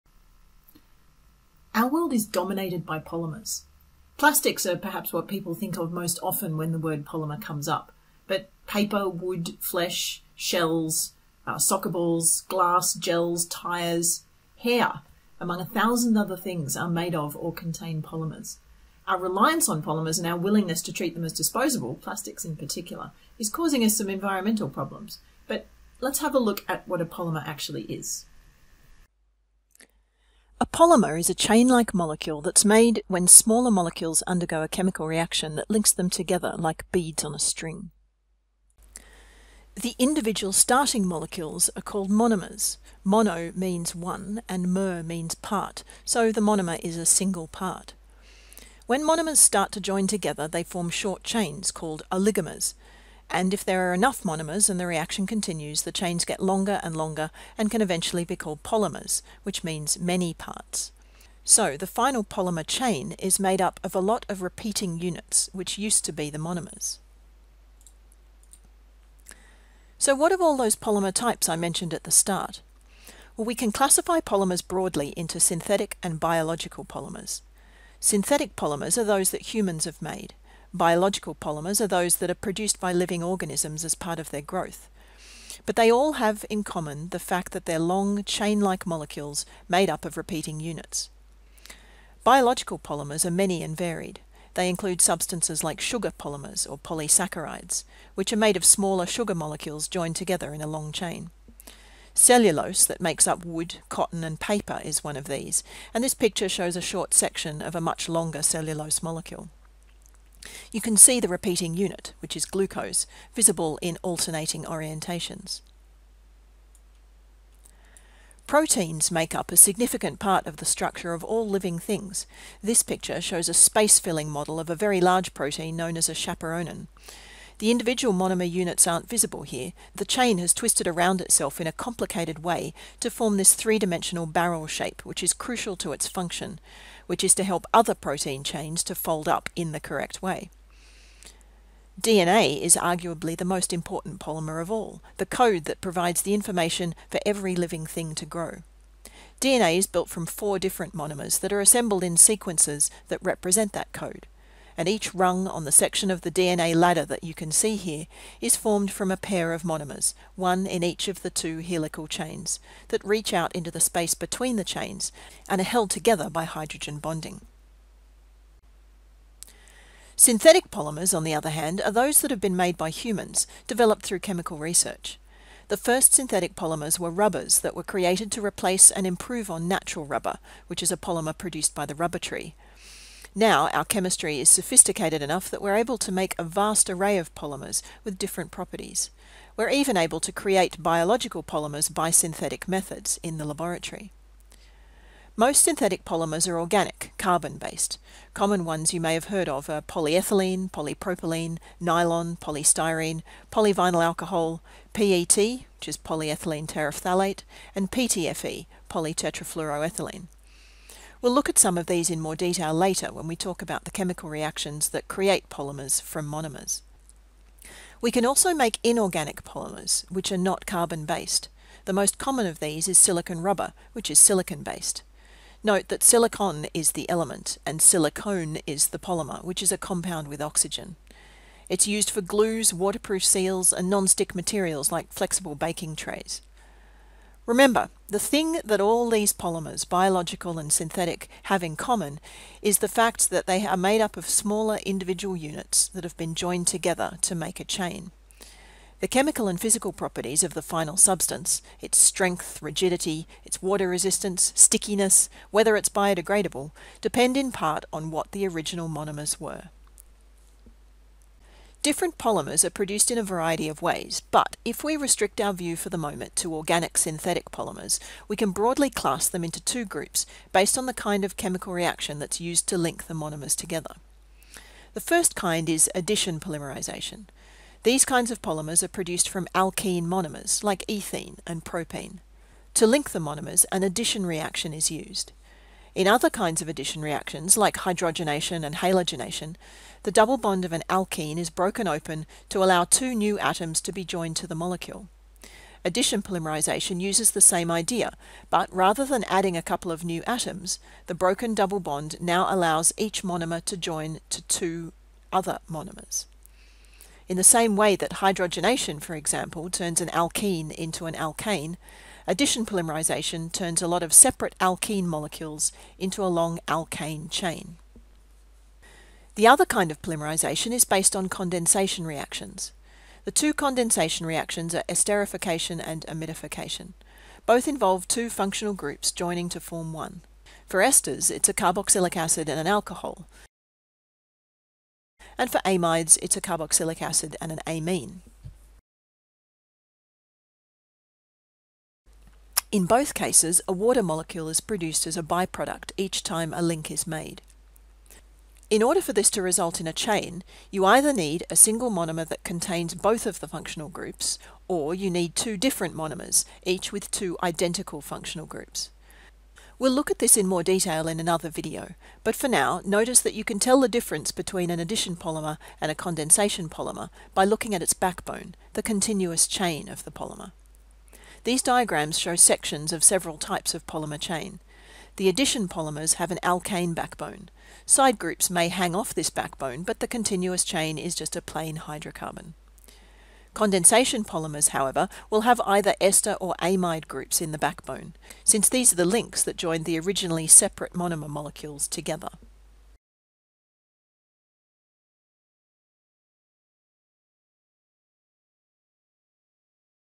What is a polymer?
Introduction to polymers; synthetic and biological polymers; addition and condensation polymers; basic polymer structure.